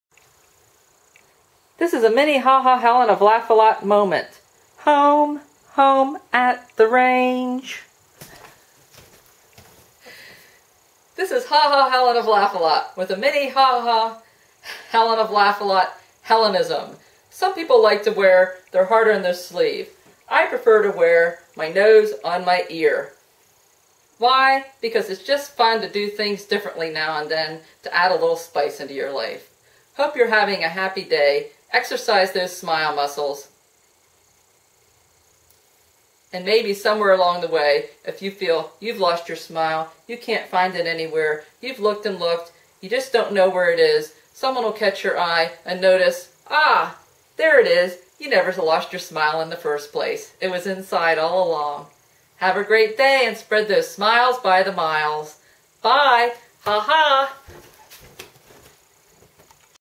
Nose on Ear
mini Ha Ha Helen Szollosy of LAFOLOT moment.  A Ha Ha Helenism: some people wear their heart on their sleeve; I wear my nose on my ear! Share a smile and help someone else find theirs! Brighten someone's day  - do things a little differently for a change of health!